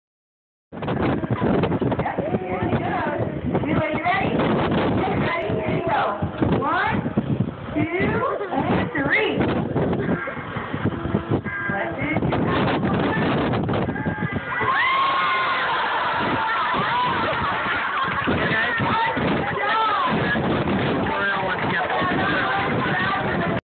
Volunteer Gets Splashed
They asked for a volunteer and this girl got picked.  Splashes her right on too.